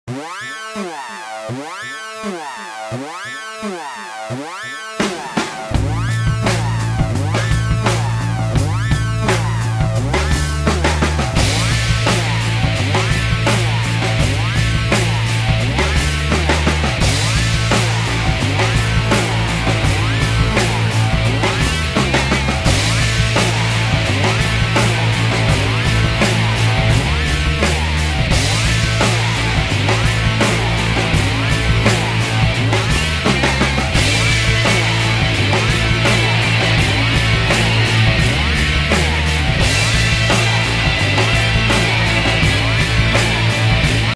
New 2014 Harley Davidson CVO Road King Motorcycles for sale
New 2014 Harley Davidson FLHRSE CVO Road King Motorcycles for sale. For more information on Price, Colors, Specs and Reviews

Featuring: upgraded bars, ABS, security, cruise control. Sherwood Green / Galactic Black (accessories and options total over $1100)

PCB
Stock# C14418Y